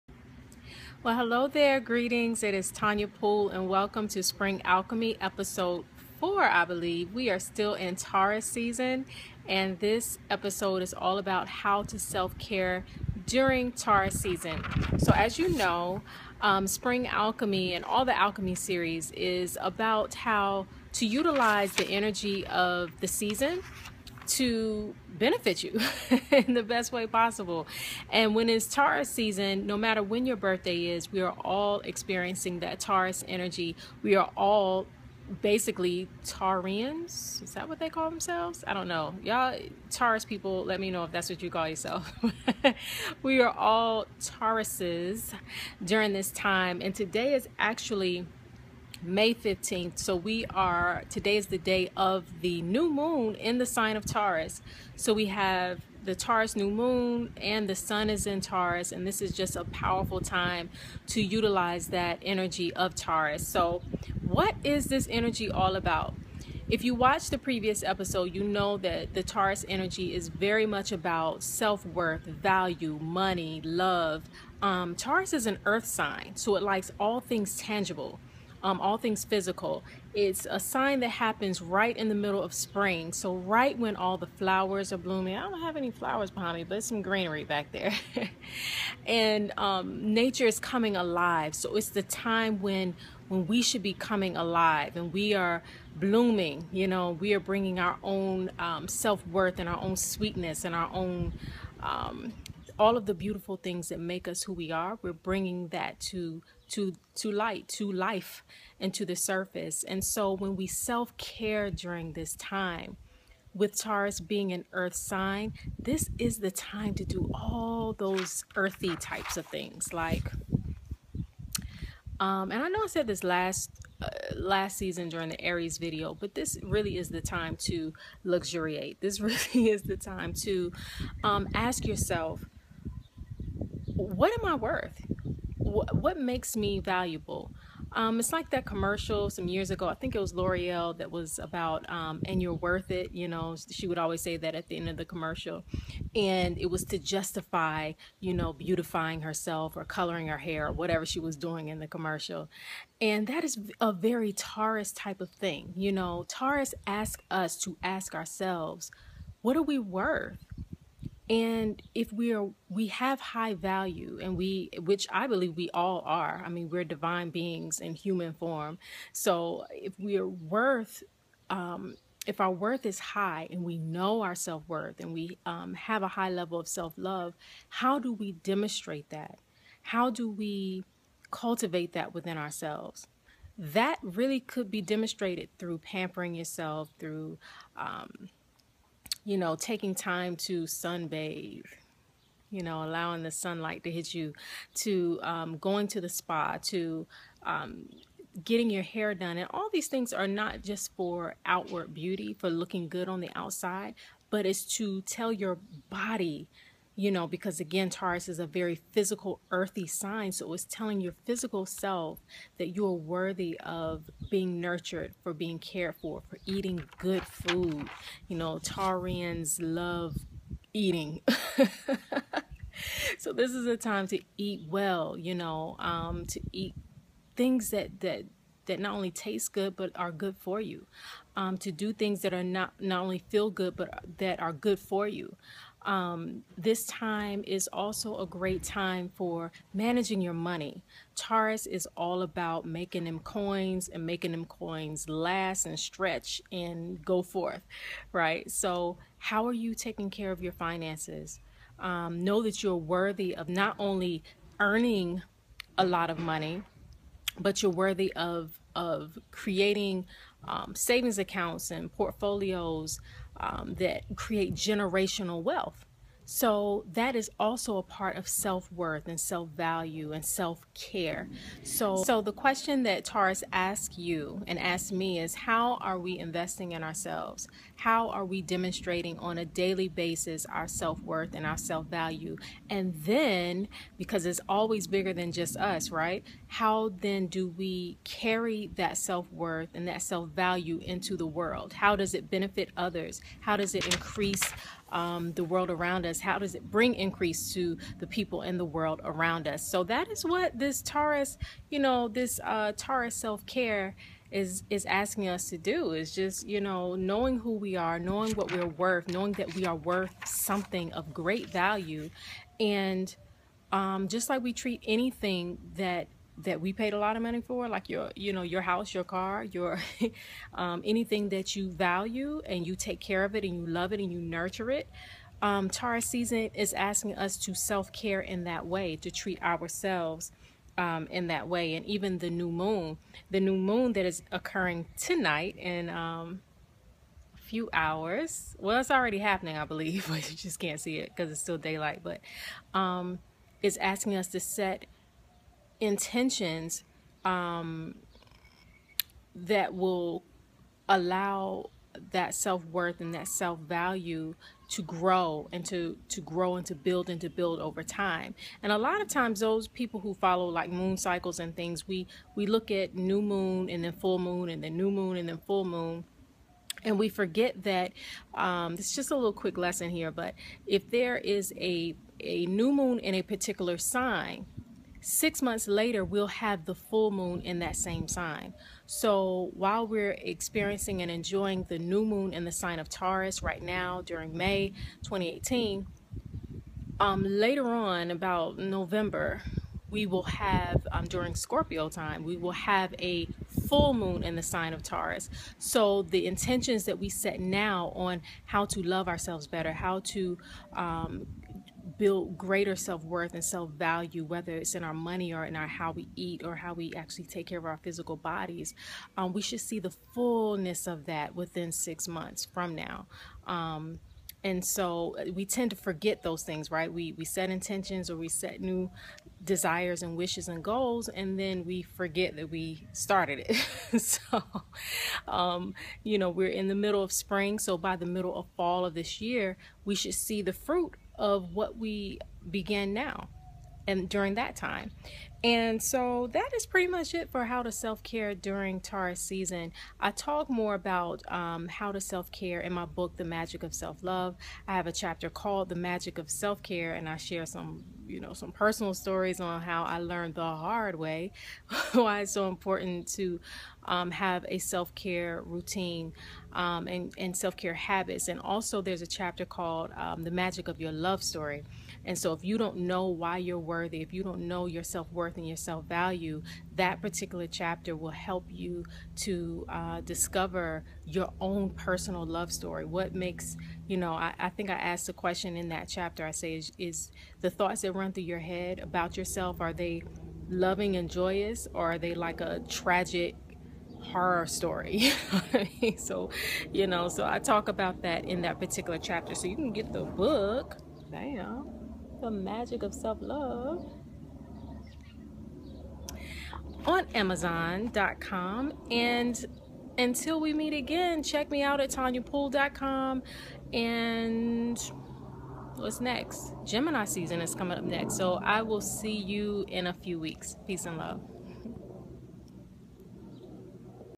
How to Self-Care during Taurus Season: Spring Alchemy Ep. 4 (2018)
Taurus is an Earth sign, which means it loves all things tangible and material. Self-care during Taurus season involves nurturing our physical bodies and our bank accounts. Watch to learn more about exactly how to self-care during this time of year. No matter when your birthday is, we are all being influenced by Taurus energy right now. Enjoy!